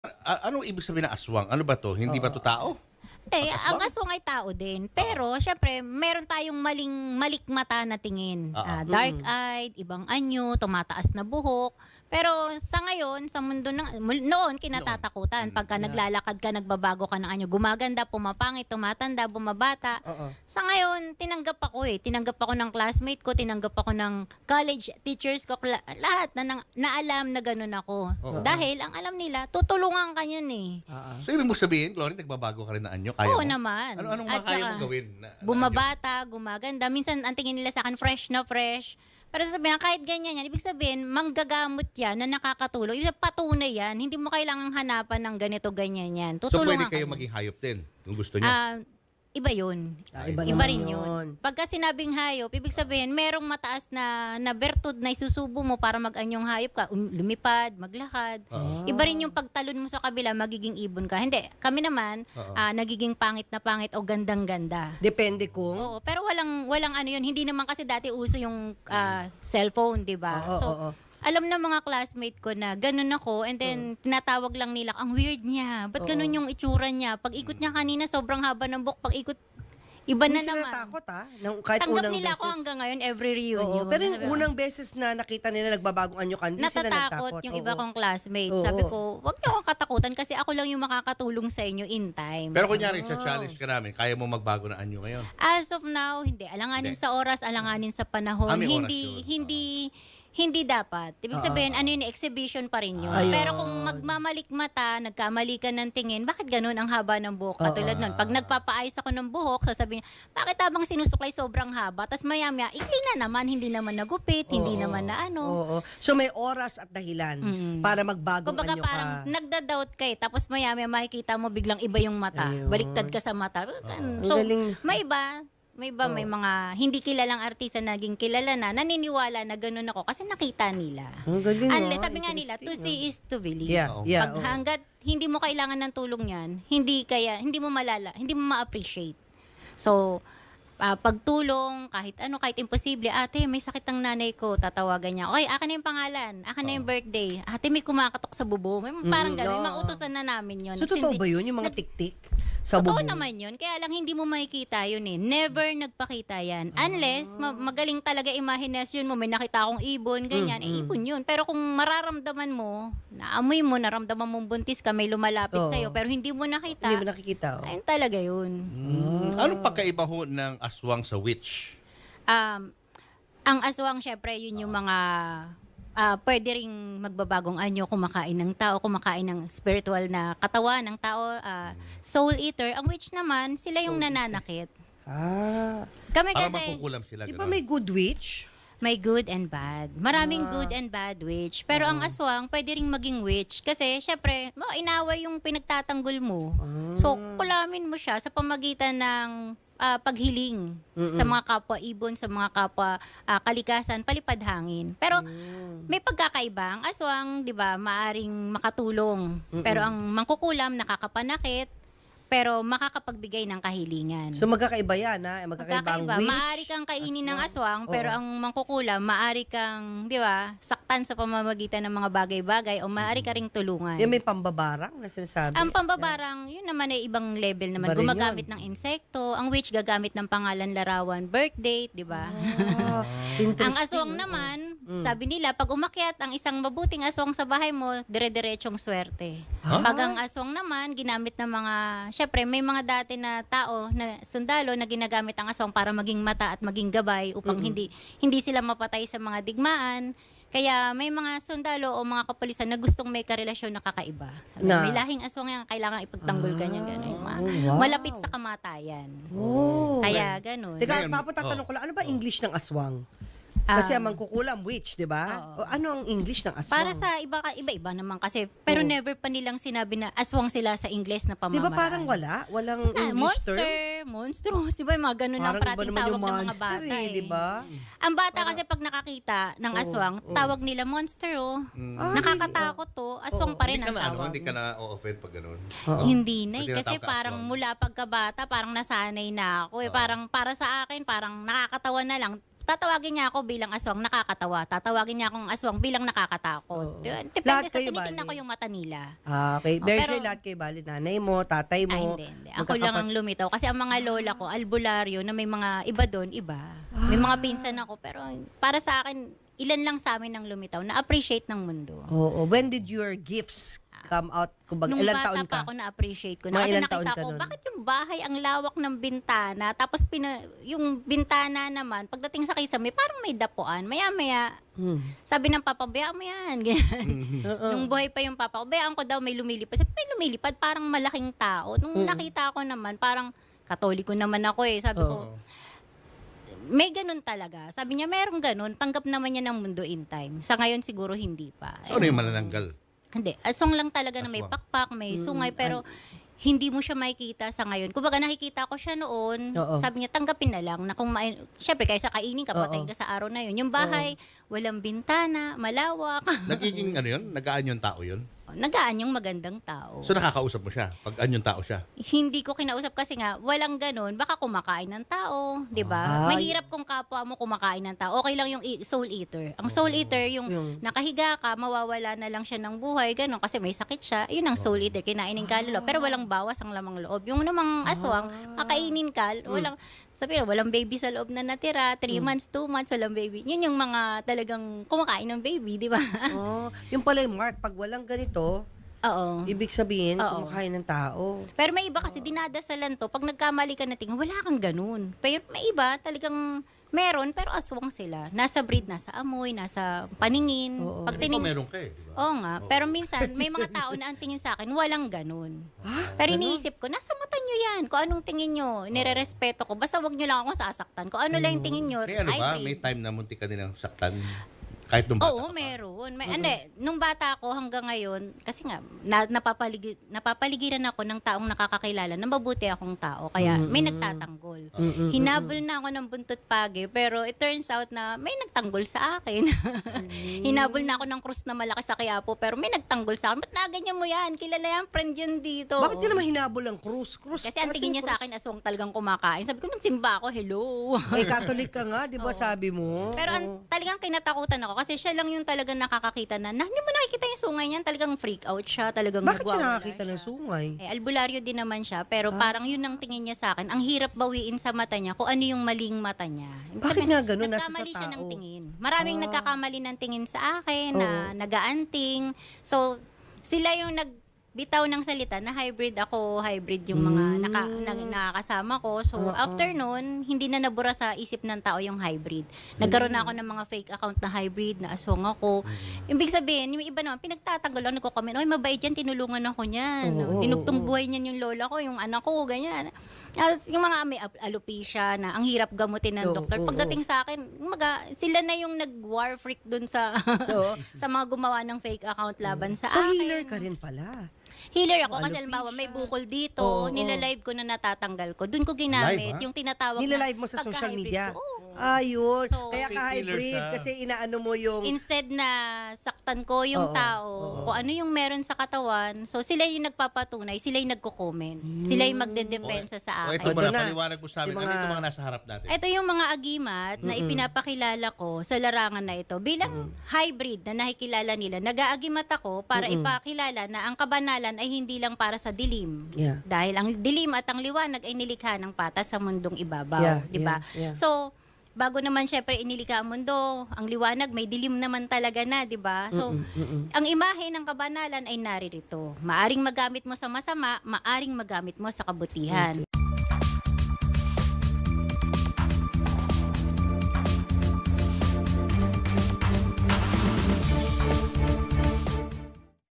Buhay ng isang babaeng aswang
Bata pa lang daw si Glorie Jane nang malaman na may lahi silang aswang. Bilang isang aswang, kaya raw n'yang magbago ng anyo. Kaya marami raw ang natatakot sa kanya sa kanilang lugar. Pero kahit isa raw siyang aswang, hindi siya nananakit ng tao. Aniya, nanggagamot pa siya ng mga may sakit.